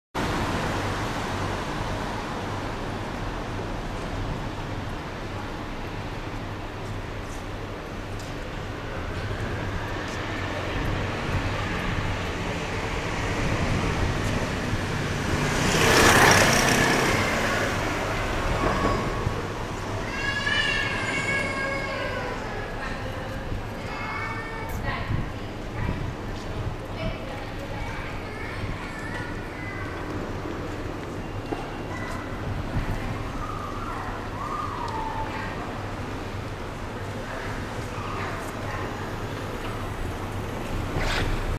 ITALIA ／MILANO 128／ミラノ
イタリア ミラノの魅力をお伝えします。人気スポットや観光名所、市街地や郊外などの映像が満載です。